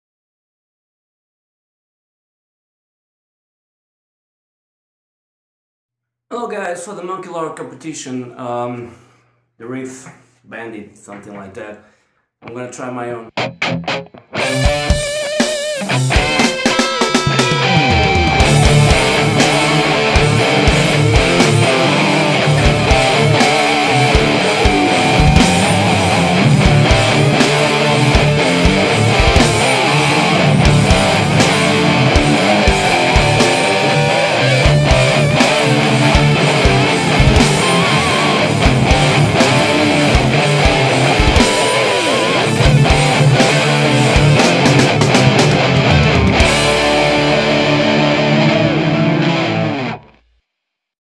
Give me a riff competition - Juca entry
My entry, hope you guys like it. As always, first take and its done. I didn't even think about what I was playing...lol
Cheers